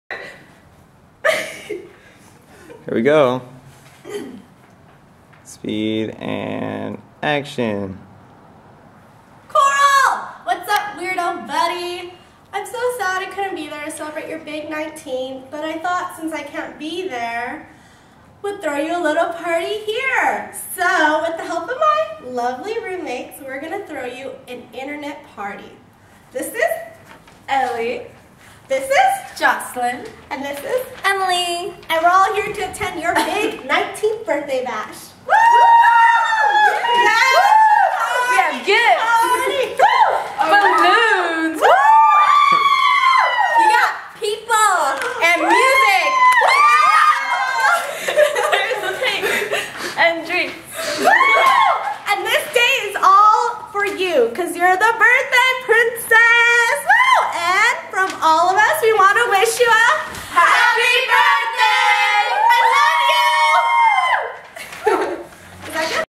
FIESTA FOR JASMIN.dv
i made this video for my weirdo sister coral since i wasn't in bakersfield to celebrate her big day with her. I hope you enjoy this my weirdo haha i love you! and i would love to thank my roomies elle, jocelyn, and emily for being a part of this video and i would also love to thank jacob for making such an amazing video :) i love you guys and thank you!!